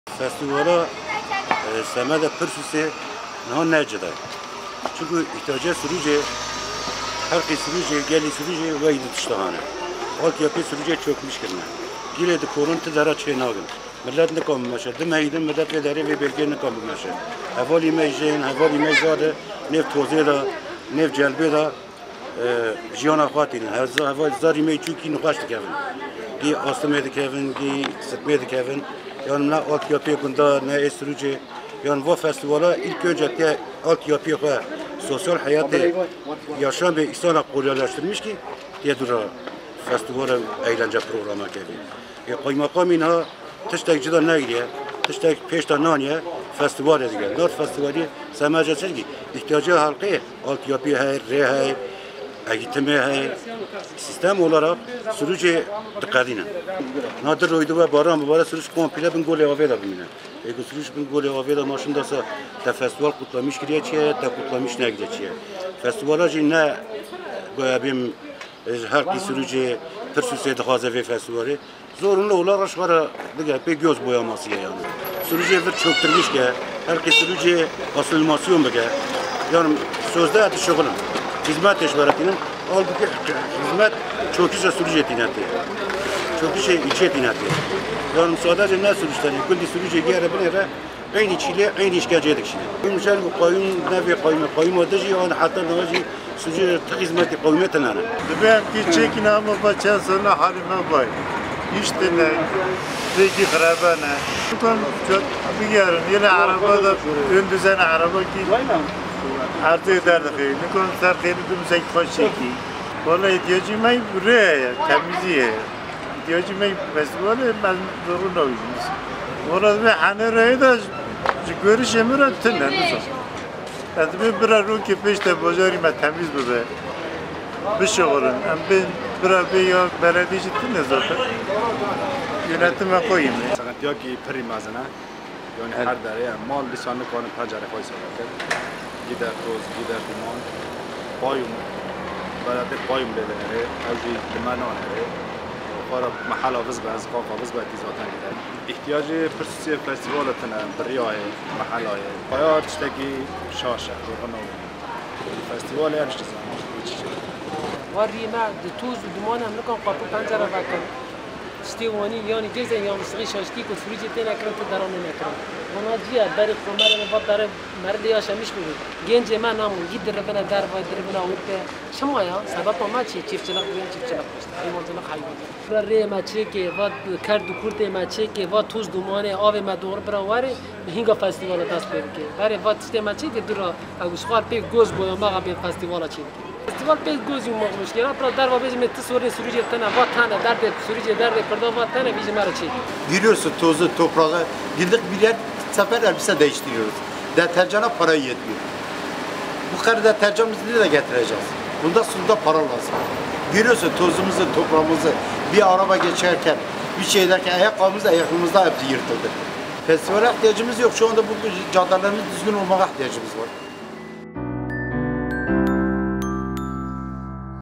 KAYYIM YOL YAPMAK YERİNE 'AT' FESTİVALİ DÜZENLİYOR
Pirsûs kayyımının köstebek yuvasına dönen yolları onarmak yerine "Suruç Atı ve Narı" festivali düzenlemesine tepki gösteren kent sakinleri, “Festivale değil, yollarımızın yapılmasına ihtiyacımız var” dedi
DAHA FAZLA BİLGİ İÇİN: